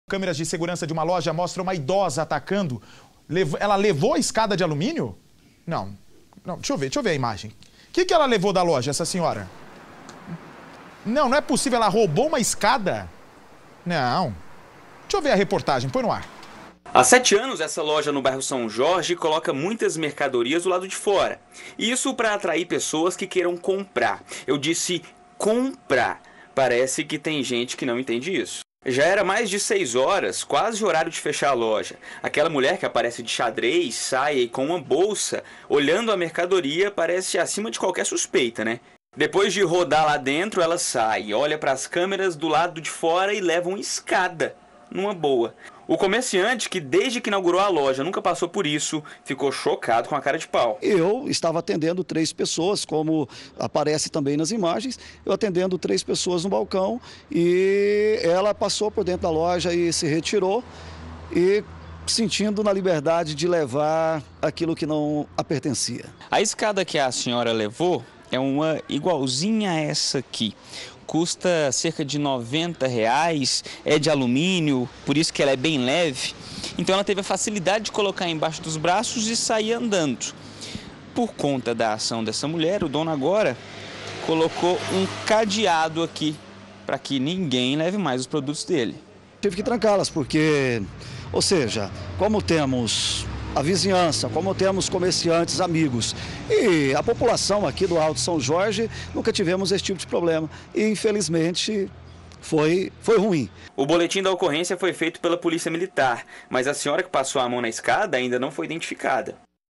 Idosa furta escada de alumínio em loja de Minas Gerais
As imagens das câmeras de segurança de uma loja flagraram o momento em que uma senhora furta uma escada de alumínio. Ela entra no estabelecimento, não compra nada e quando chega do lado de fora, pega a escada. Veja!